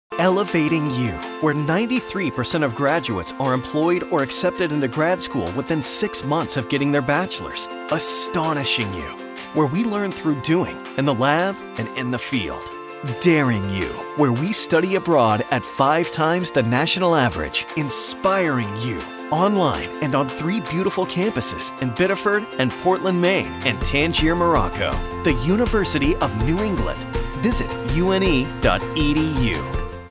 Inspiring U: The University of New England
Discover Maine’s largest private University, the University of New England. UNE offers more than 40 bachelor’s degrees in needed fields, from the health professions, to the health and life sciences, to business and the humanities. We also offer 30-plus graduate, professional and doctoral degrees in medicine, dentistry, pharmacy, social work and more. UNE learning happens online and on three beautiful campuses: on the shore in Biddeford, Maine, in-town in Portland, Maine, and at our new study-abroad campus in Tangier, Morocco. for complete information.

The University of New England is known as UNE, but call us “ELEVATING U”, because we work hard to help students launch a good career. In fact, a recent poll showed that 93% of UNE grads are employed or accepted into grad school within 6 months of earning their bachelors. Call UNE “Astonishing U”, because here, you’ll learn through doing, in fascinating research and hands-on projects. Call us “Daring U”, because our students study abroad at five times the national average, to get a global perspective. Whatever you call us, we hope to… Inspire U.